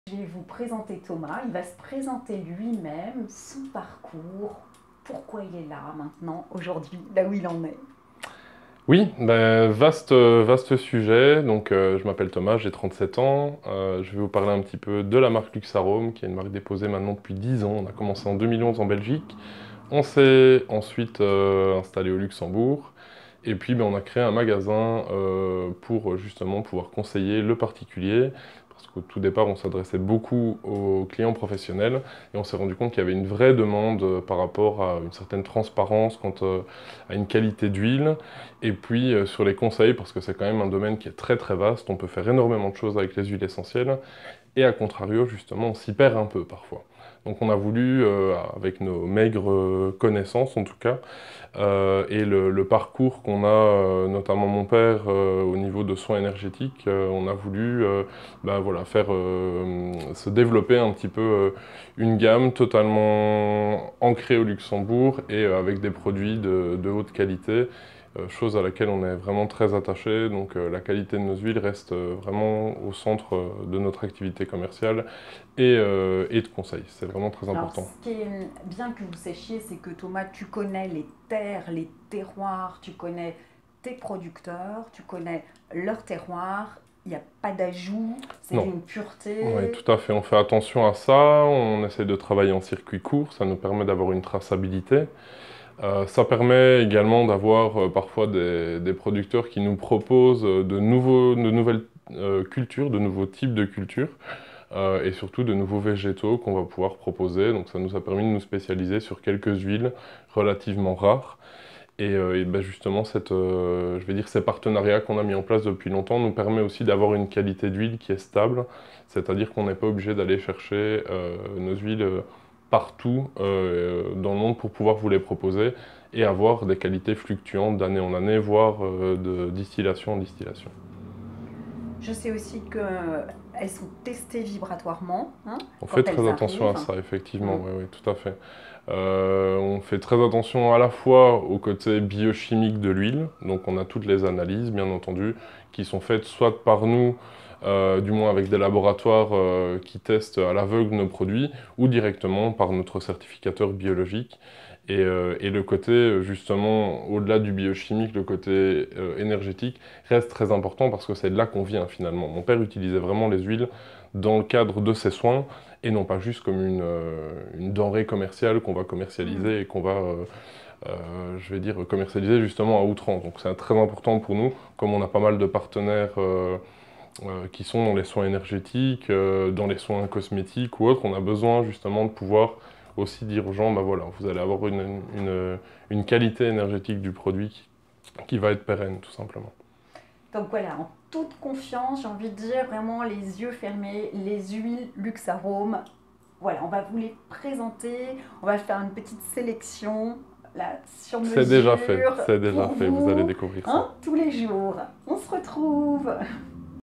Il était une fois une huile essentielle : présentation de Thomas, le maestro des huiles
Thomas de Luxaromes, passionné et passionnant nous présente dans cette série de vidéos des huiles essentielles rares, atypiques, utiles à nos vies !